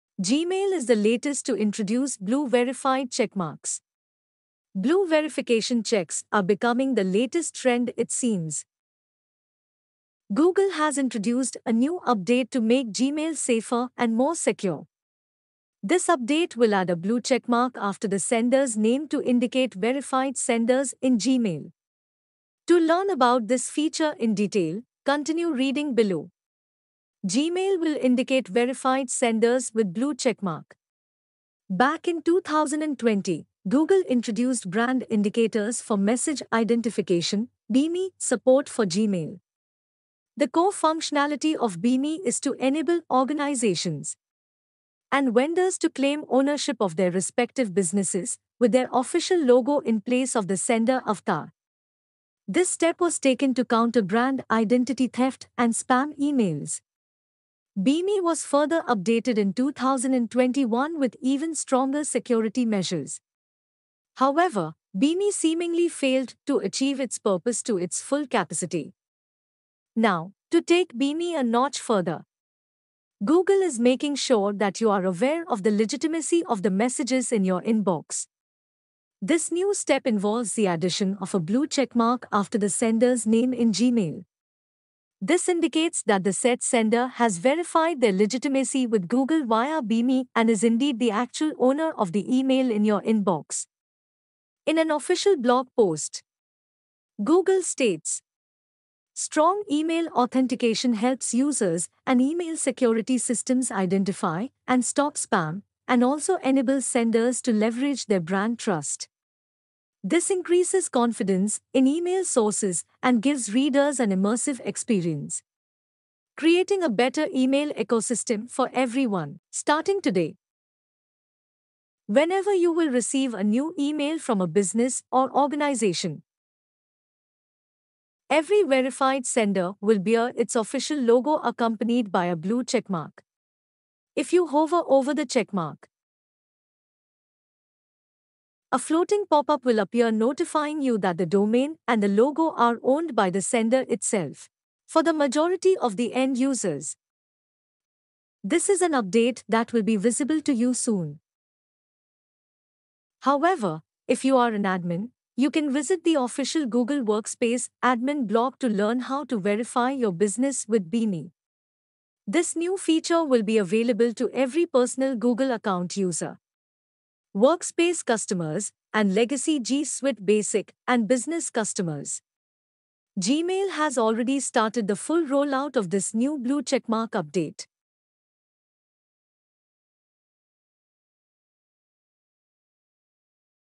Gmail is the latest to introduce blue verified checkmarks
video content beebom website.
voice credit ai voice English in India